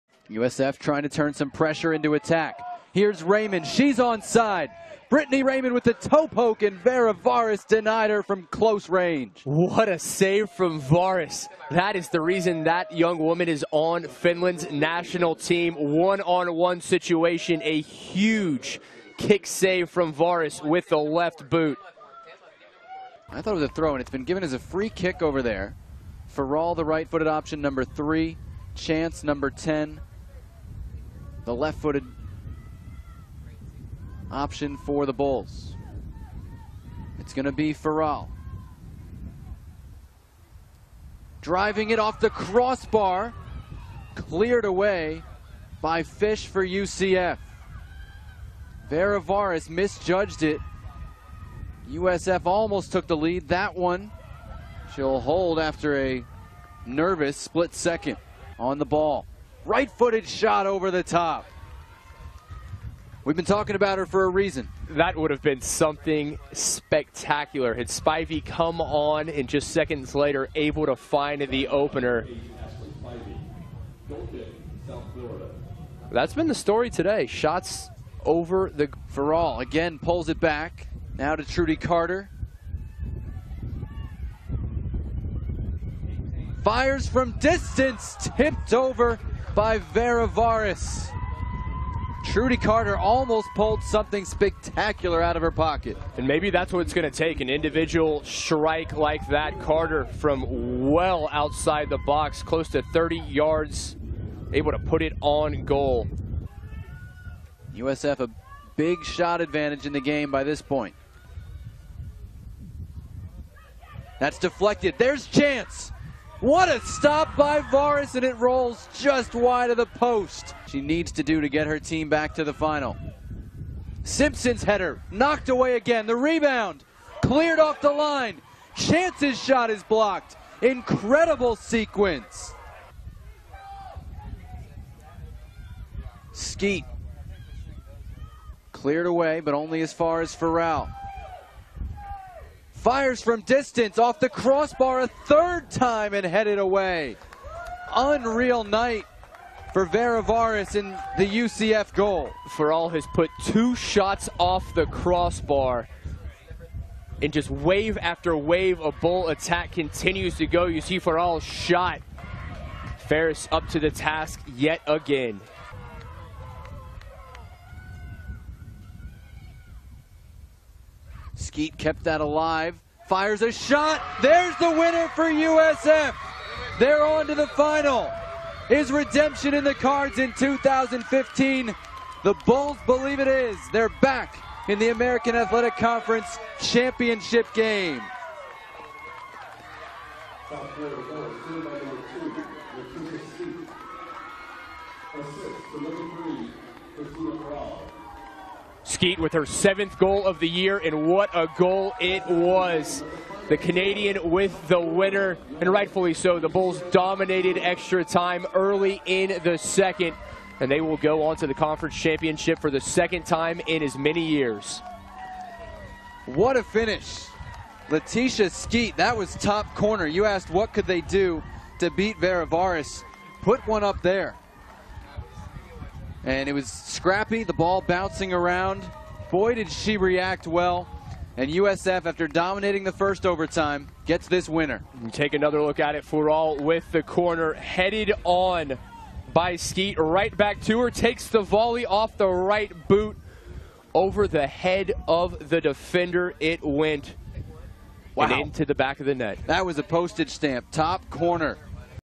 Women's Soccer Championship Highlights: USF 1, UCF 0 (2OT)
The USF Bulls advance to the the finals of the 2015 American Athletic Conference Championship with a 1-0 double-overtime win over the UCF Knights. Leticia Skeete scored the winning goal.